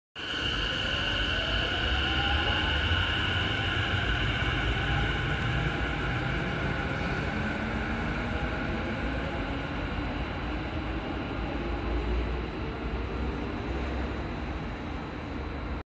JR横浜線
町田から新横浜駅まで乗りました。車体に葉っぱのマーク(シンボル)🍃がありました。